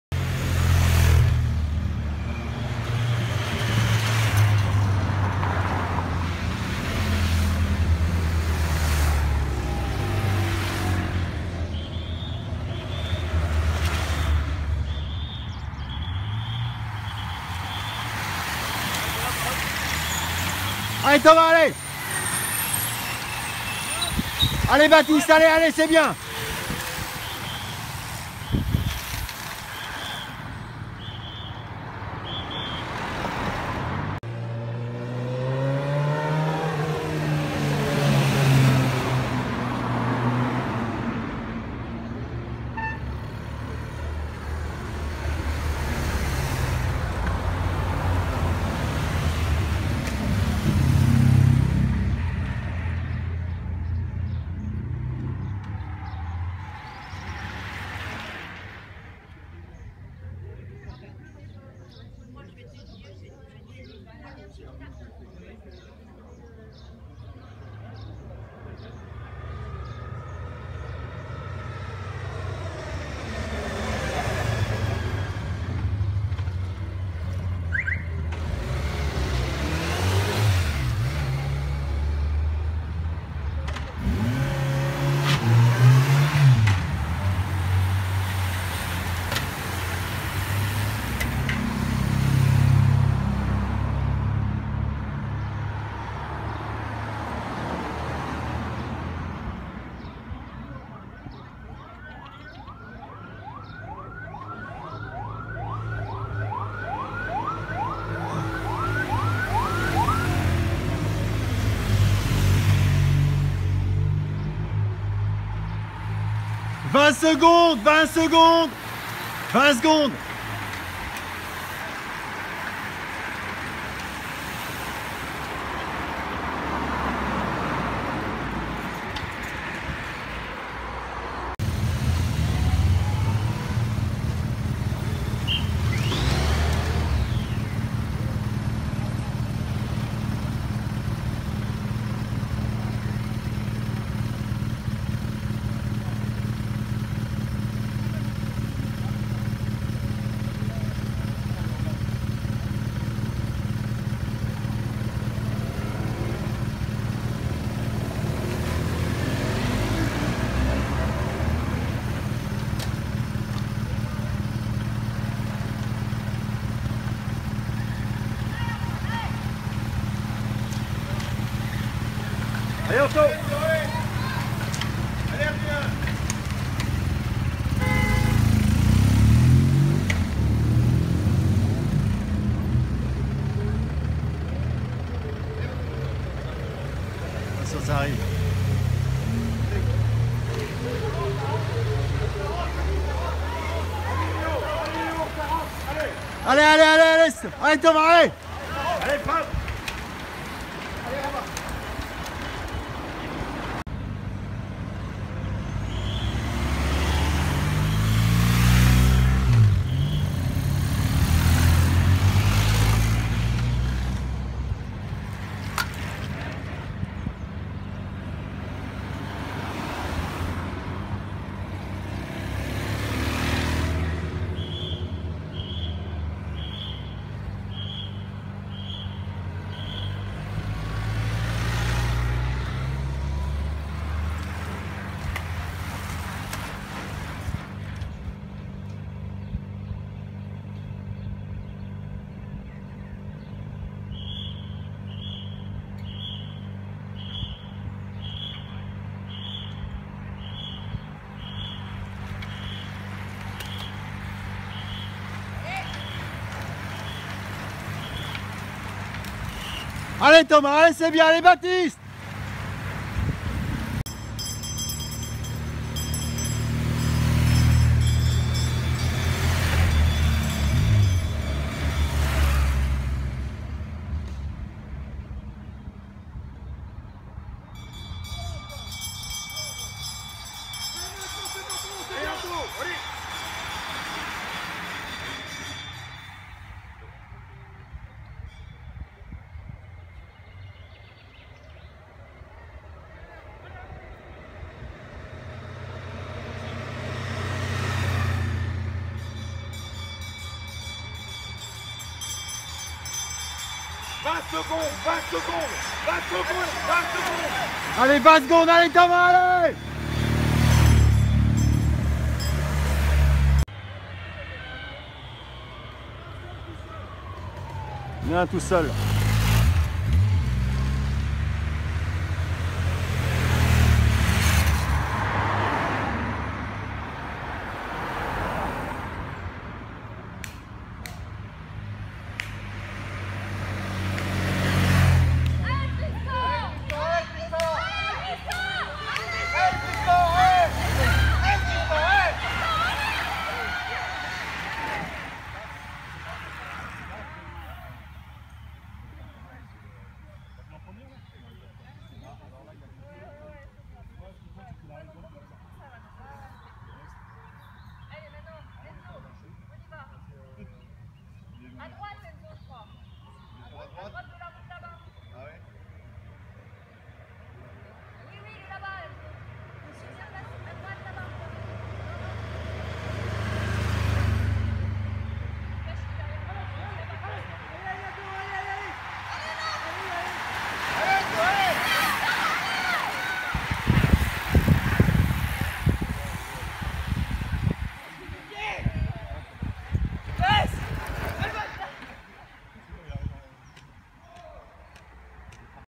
Course de Villepreux - Gambais en 3,J et Open
Course de Villepreux - Gambais en 3,J et Open avec Thomas REMY et Baptiste Soluch